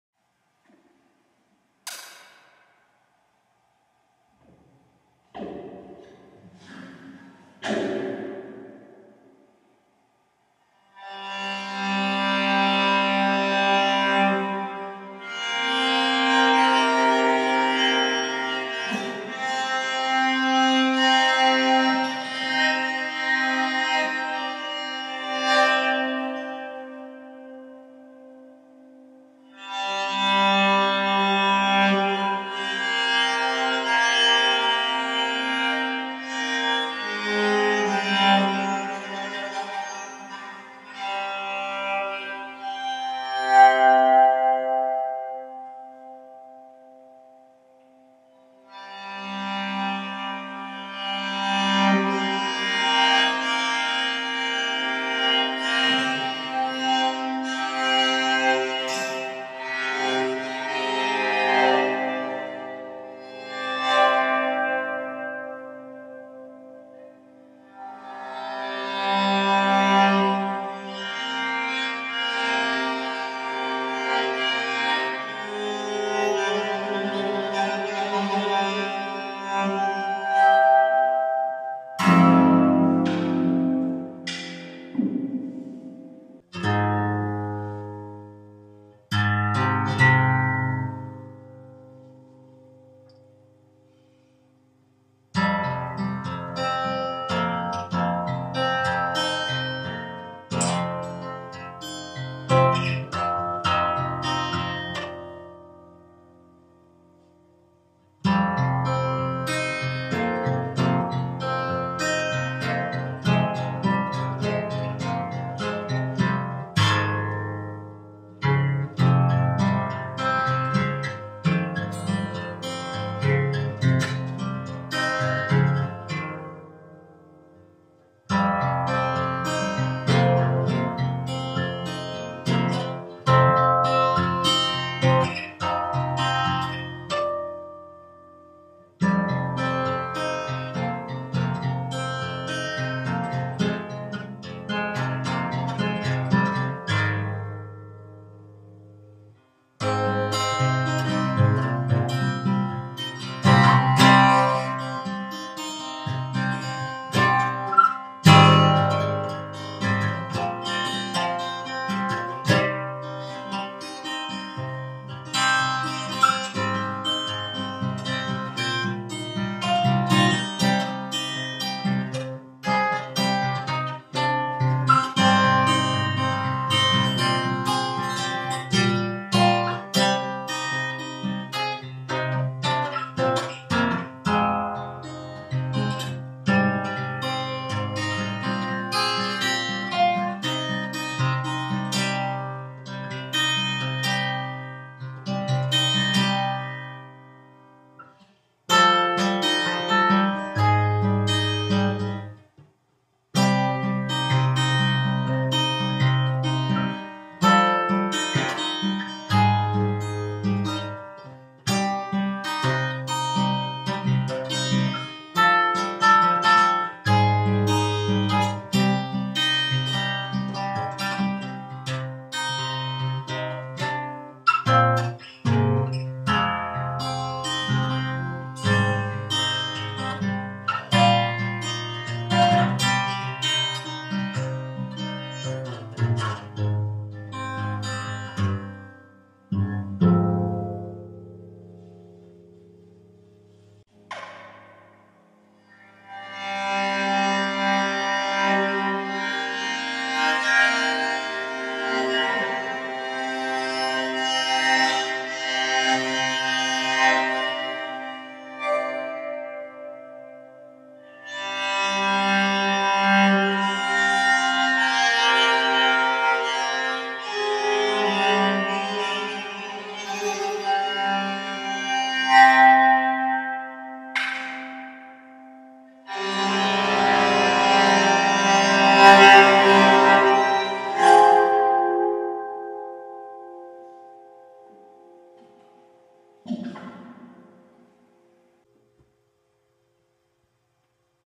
Cello Guitar
Made a song using my violin bow to make my guitar kind of like a cello.